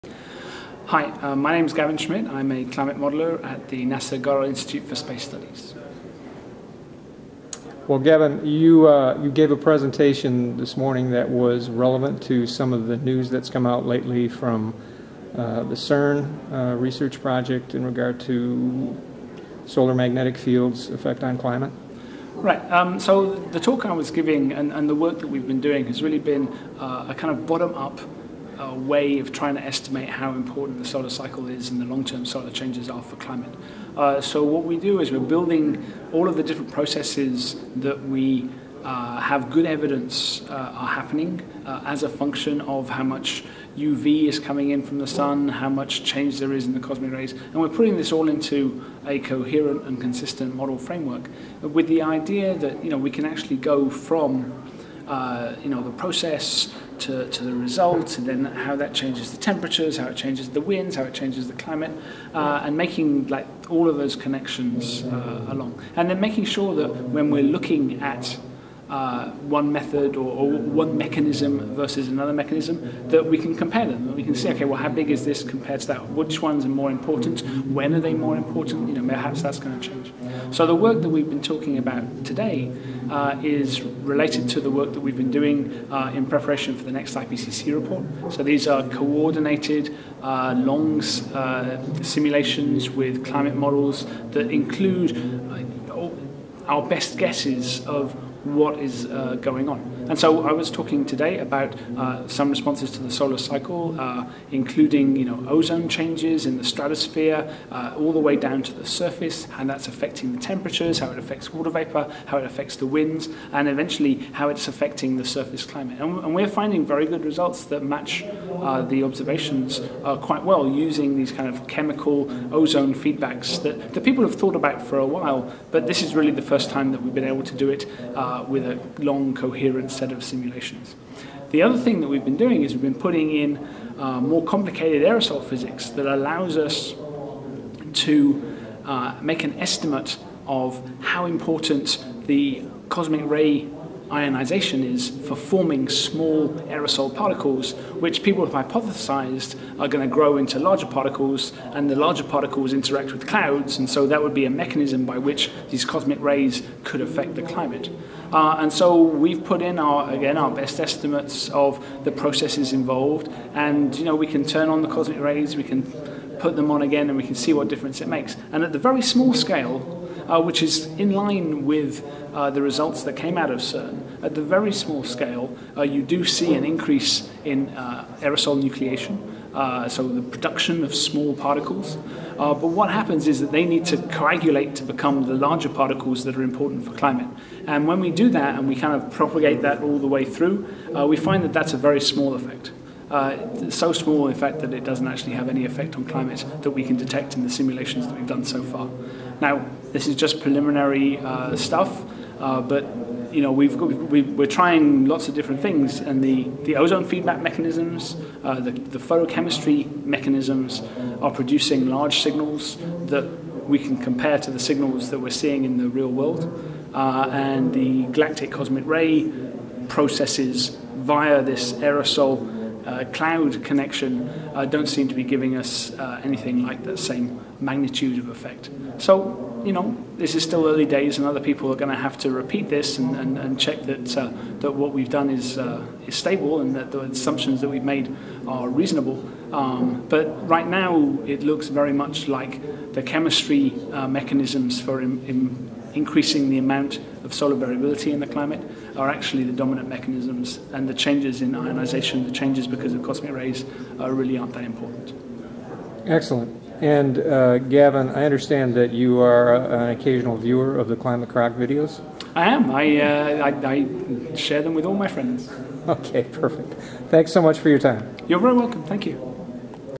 Gavin Schmidt at American Geophysical Union
NASA Atmospheric Scientist Gavin Schmidt gives brief synopsis of his presentation at the American Geophysical Union - including new perspectives on the CERN cosmic ray results.